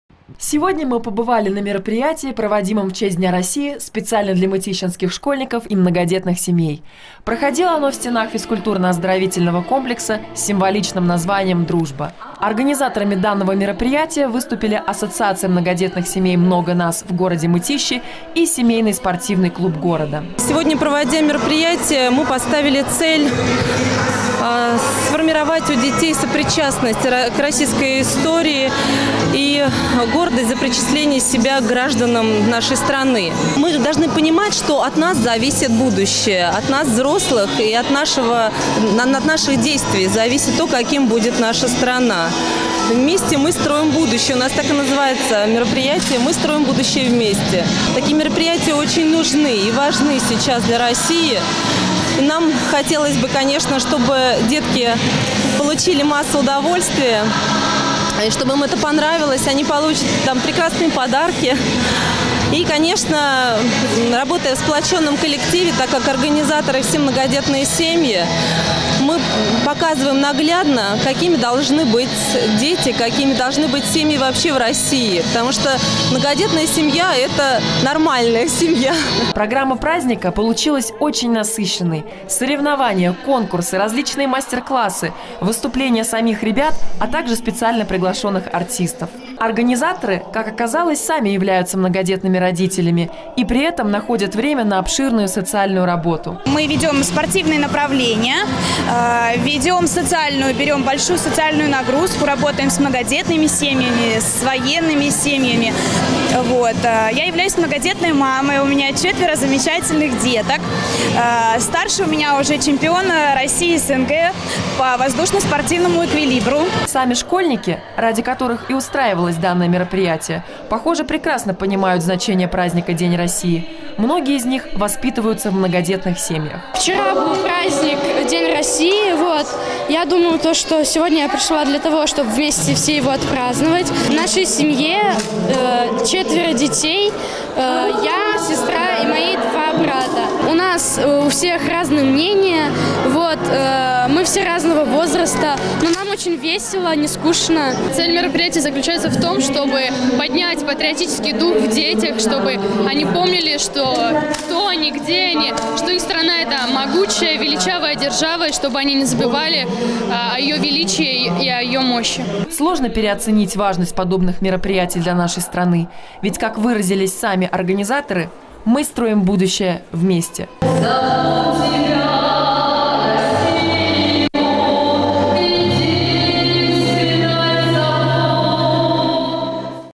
Праздник в ФОК «Дружба»
Праздник День России уже прошел, но мероприятия приуроченные к нему еще проводятся в нашем округе и в них с удовольствием принимают участие как взрослые так и дети, одно из них прошло в физкультурно-оздоровителньом комплексе с символичным названием «Дружба».